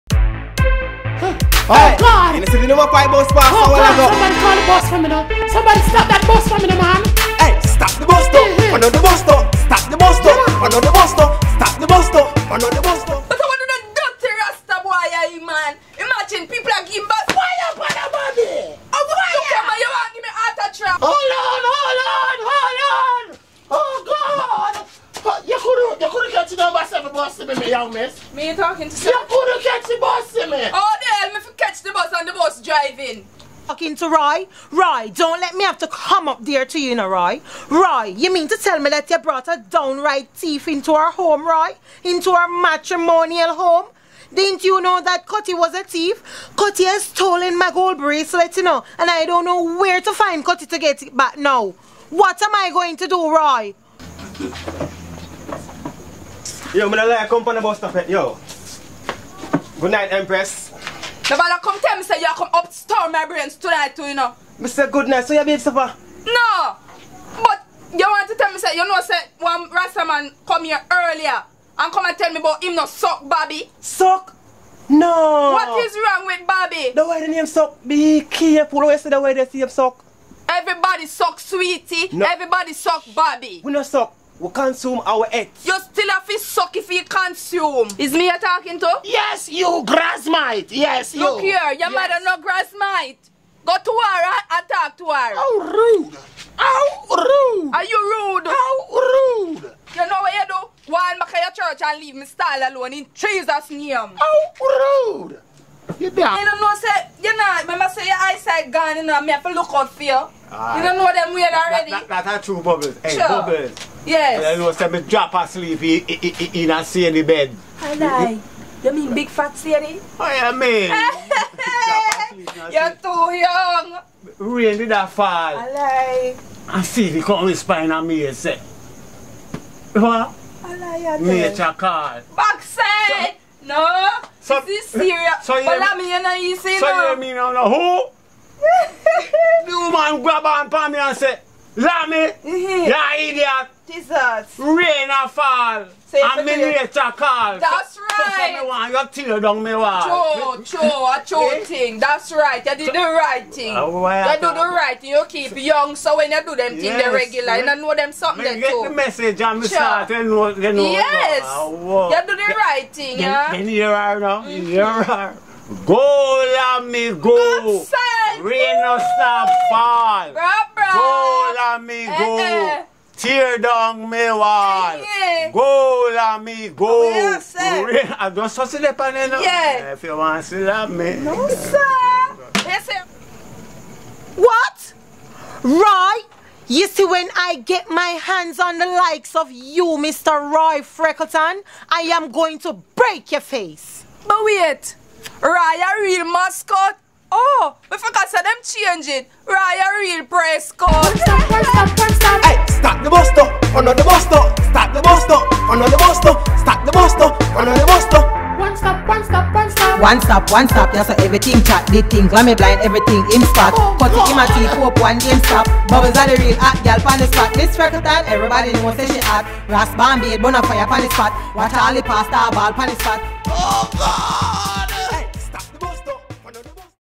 One stop Play Trailer
its a bus stop settings where everything takes place, done in Jamaica
coming soon to Canada, England and America